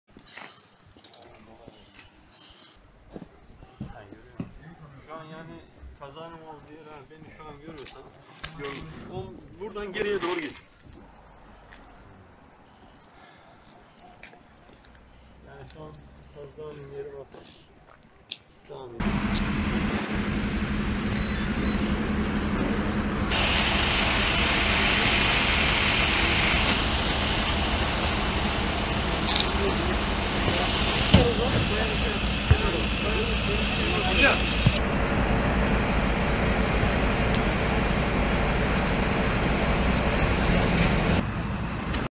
Bursa'da feci kaza! İki otomobilin çarpışması sonucu 5 kişi yaralandı
Bursa'nın İnegöl ilçesinde, iki otomobilin çarpıştığı kazada 1'i ağır 5 kişi yaralandı.